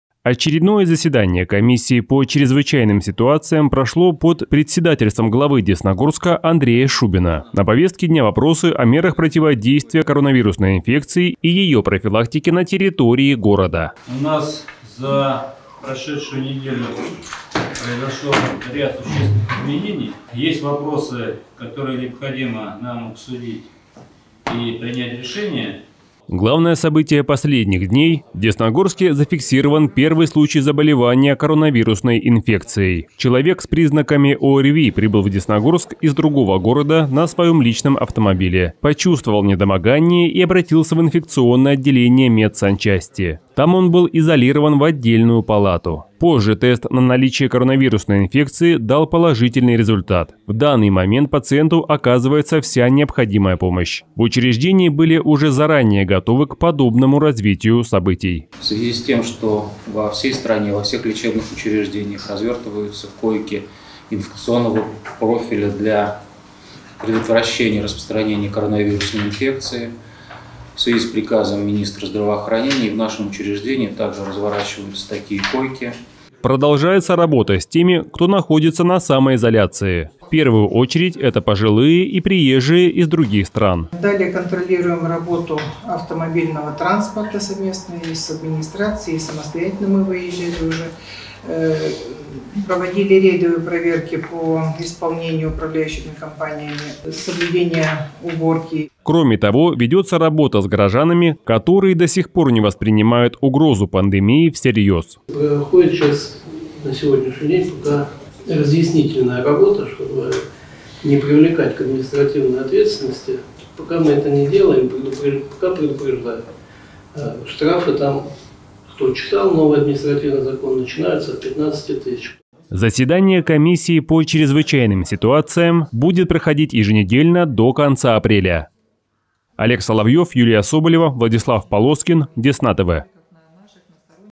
Десна-ТВ: Заседание комиссии по чрезвычайным ситуациям
7 апреля прошло еженедельное заседание комиссии по чрезвычайным ситуациям. На повестке дня вопросы, так или иначе касающиеся эпидемической ситуации в Десногорске.